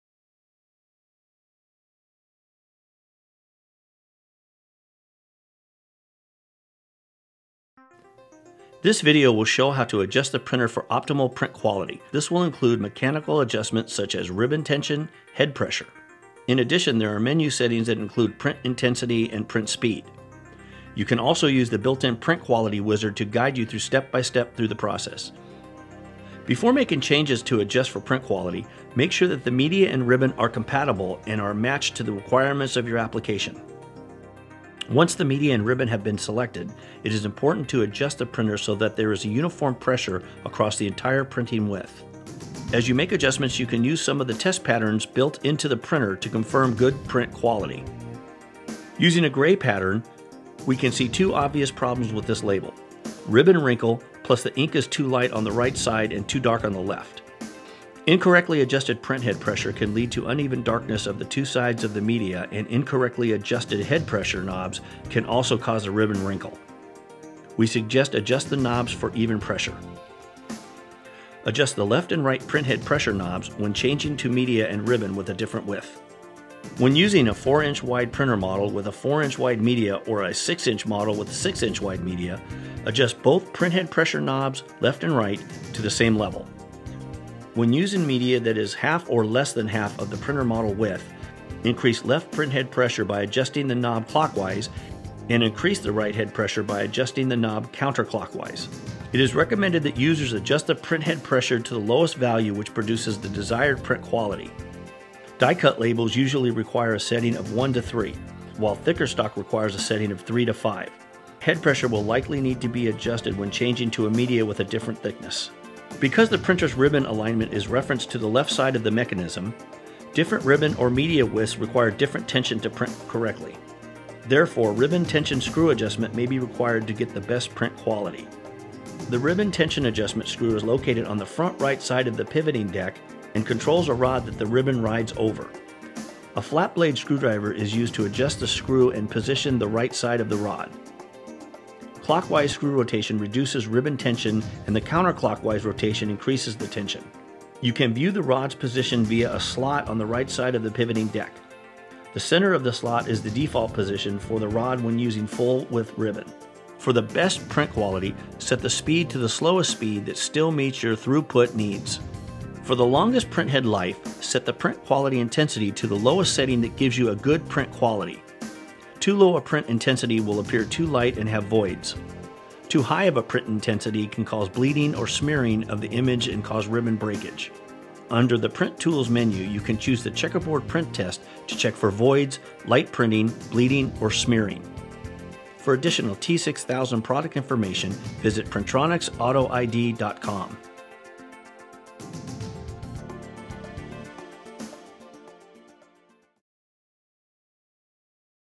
Printronix Auto ID T6000 Print Quality
How to adjust the Print Quality on your T6000 printer from Printronix Auto ID.